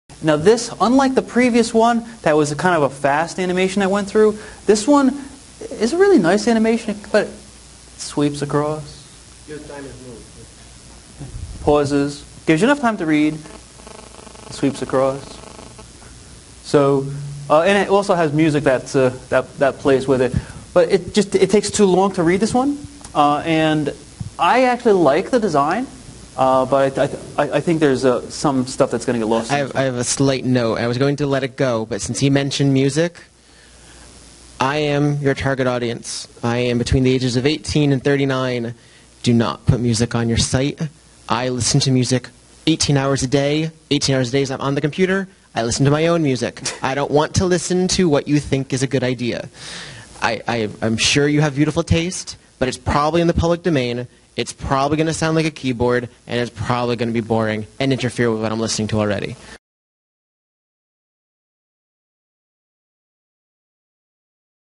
Music on a Jewelry Website
Honestly, if another person asks us to put music on a jewelry website we just might scream.  You might think it makes your website interesting and cool, but in reality music on a jewelry website is akin to website black plague. This is an excerpted clip from the retail jewelry store website training we provide.  For the next live jewelry website training event please